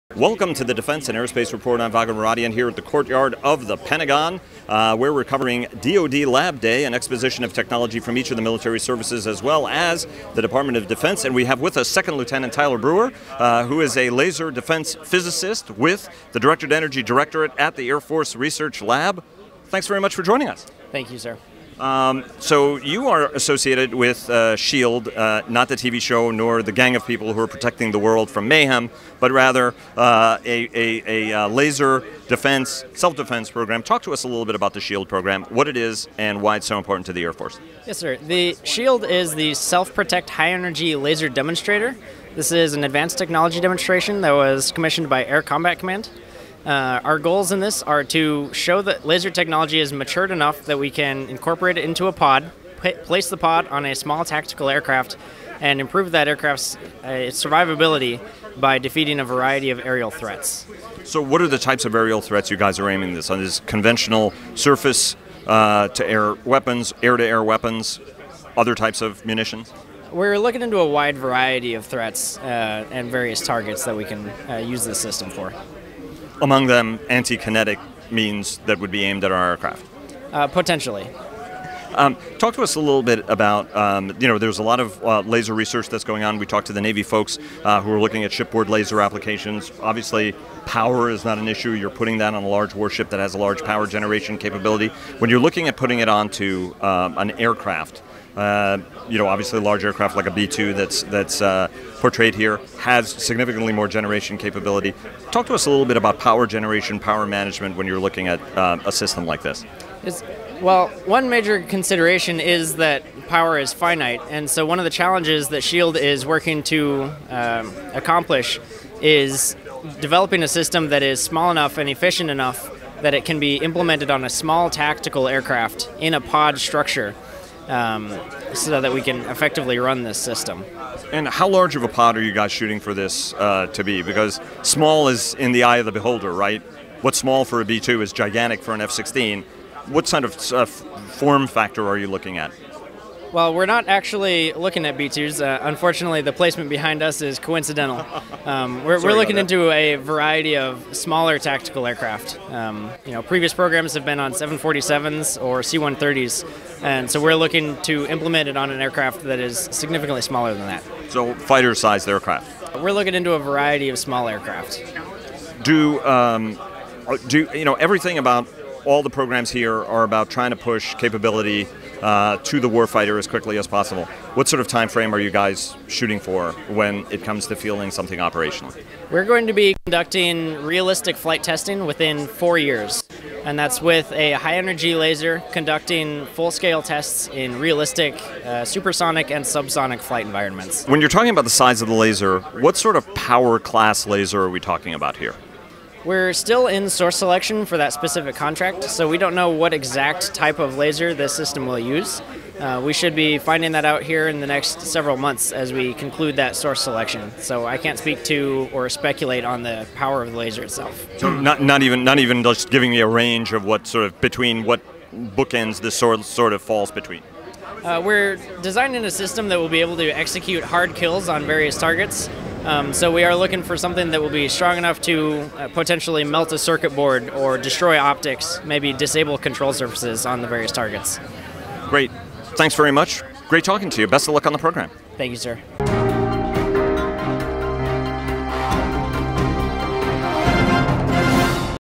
USAF's Brewer on Improving Aircraft Survivability with Lasers
2nd Lt. Tyler Brewer, USAF, a laser physicist with the US Air Force Research Laboratory's Directed Energy Directorate at Kirtland Air Force Base, N.M., discusses how the lab's SHiELD (Self-Protect High Energy Laser Demonstrator) program can defend aircraft with Defense & Aerospace Report Editor Vago Muradian at DoD Lab Day 2017 at the Pentagon on May 18, 2017. SHIELD aims to explore the potential of lasers in pods that can be mounted on future aircraft.